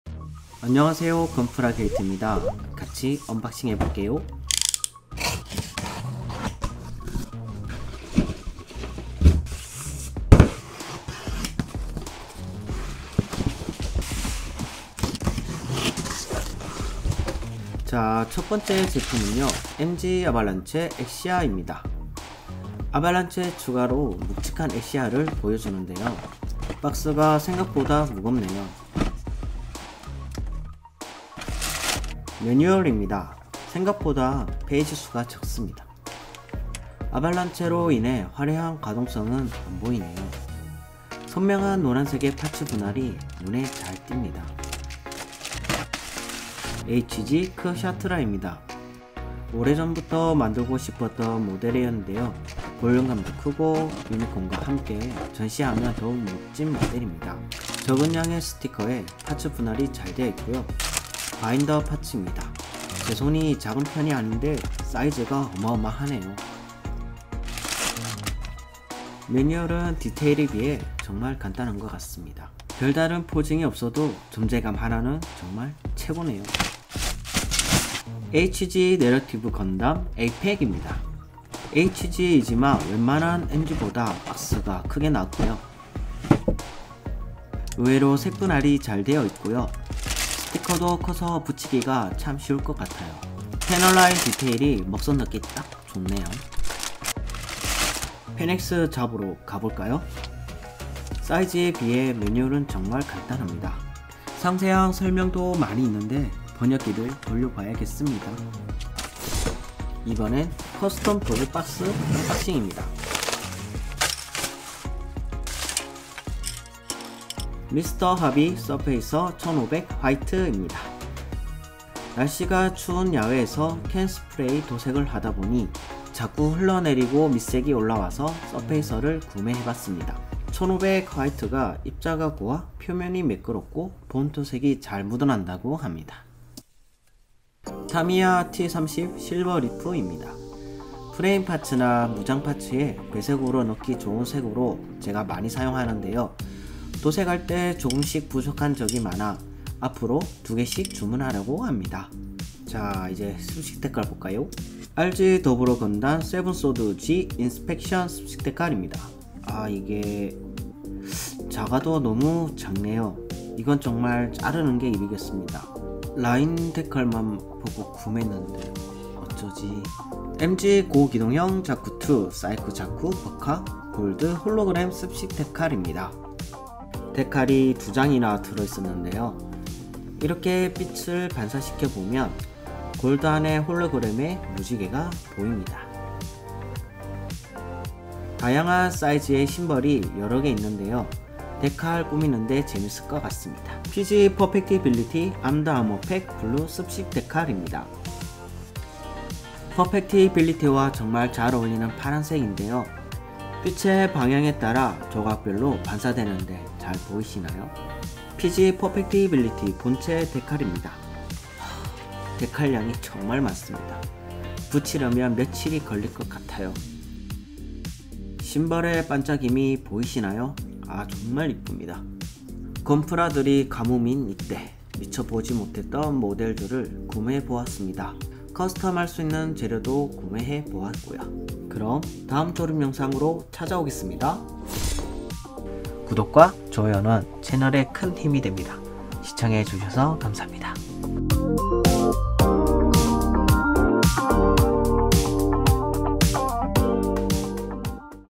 건프라 살게 없다고?! 그래도 산다! 반다이 언박싱과 커스텀 도구 구매 언방식편
안녕하세요. 건프라 게이트입니다.
건담베이스 안간지 오래 되었네요. (한달에 두번 이상은 갔었는데 ㅜㅜ)
그동안 메인 프라모델에 가려져 있었지만 멋진 모델들 찾아서 구매 했습니다.
다같이 언박싱 해보겠습니다.

MG 아발란체 엑시아
HG 크샤트리아
HG 내러티브 건담 A 팩

RG 00 건담 세븐 소드/G 인스펙트 (습식데칼)
MG 고기동형 자쿠2 (싸이코 자쿠) Ver.Ka (골드홀로그램 습식데칼)
PG 퍼펙티 빌리티 (블루 자개_홀로그램 습식데칼)

타미야 캔 스프레이 T-30 (실버리프)
Mr.hobby 서페이서 1500 화이트